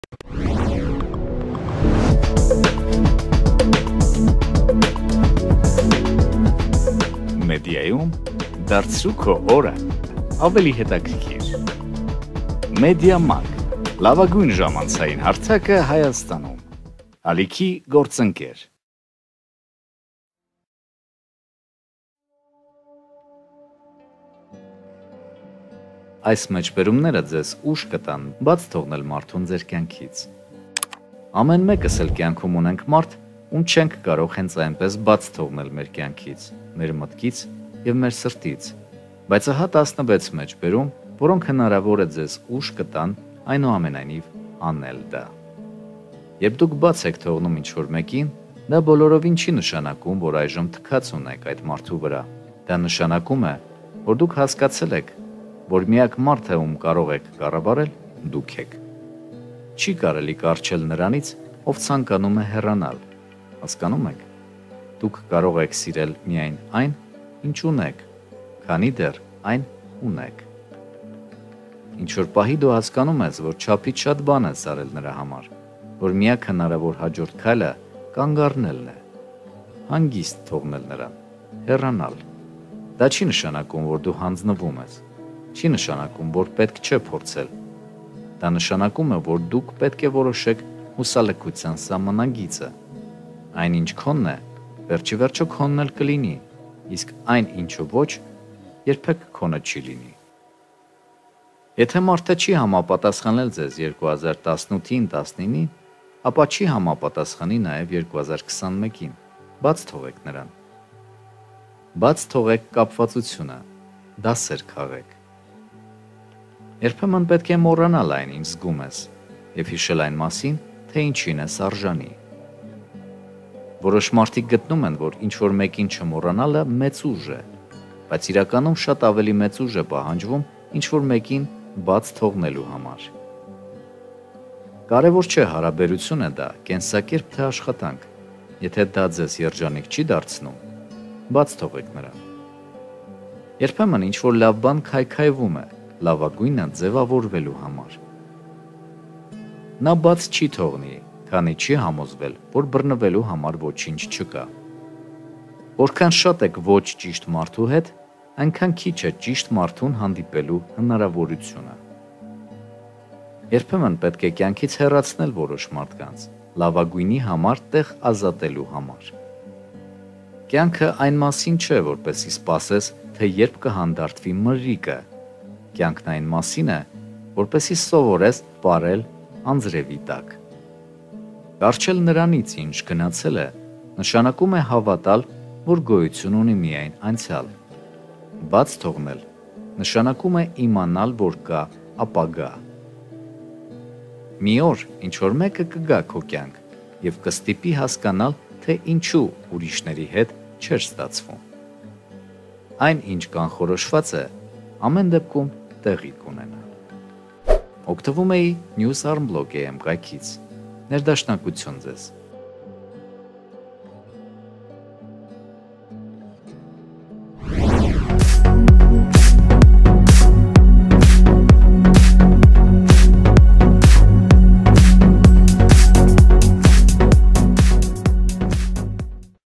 Այս խոսքերը քեզ ուժ կտան՝ բաց թողնել մարդուն քո կյանքից
Ամեն մեկս էլ կյանքում ունենք մարդ, ում չենք կարող հենց այնպես բաց թողնել մեր կյանքից, մեր մտքից և մեր սրտից: Բայց ահա 16 մեջբերում, որոնք հնարավոր է ձեզ ուժ կտան այնուամենայնիվ անել դա:

1. «Երբ դուք բաց եք թողնում ինչ-որ մեկին, դա բոլորովին չի նշանակում, որ այժմ թքած ունեք այդ մարդու վրա: Դա նշանակում է՝ դուք հասկացել եք, որ միակ մարդը, ում կարող եք կառավարել՝ դուք եք»:

2. «Չի կարելի կառչել նրանից, ով ցանկանում է հեռանալ, հասկանու՞մ եք: Դուք կարող եք սիրել միայն այն, ինչ ունեք, քանի դեռ այն ունեք»:

Շարունակությունը՝ հոլովակում: Աղբյուրը՝ News.armblog.am

Եթե դեռ բաժանորդագրված չեք «ժամանց և Տեղեկատվություն» ալիքին, կարող եք սեղմել այստեղ․